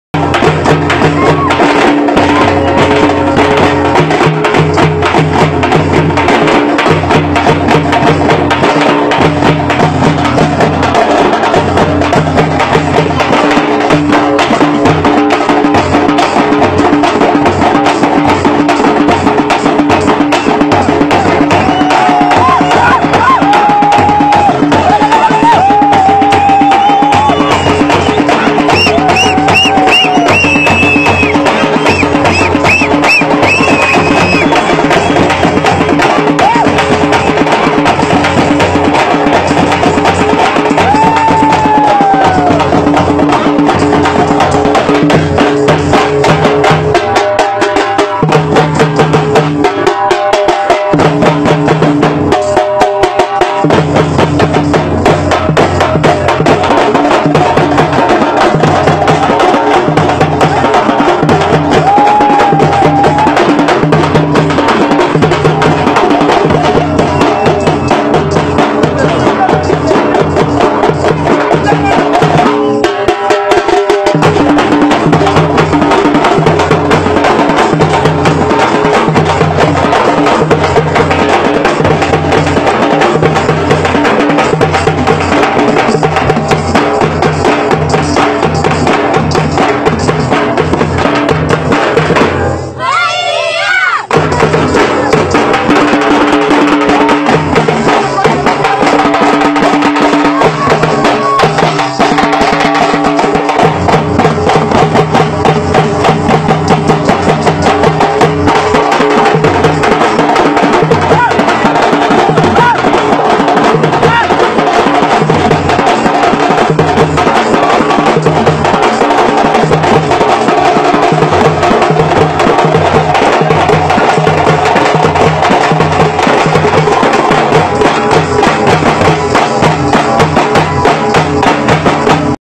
Drumming from college women's team || Karnataka || India
college students drumming in strret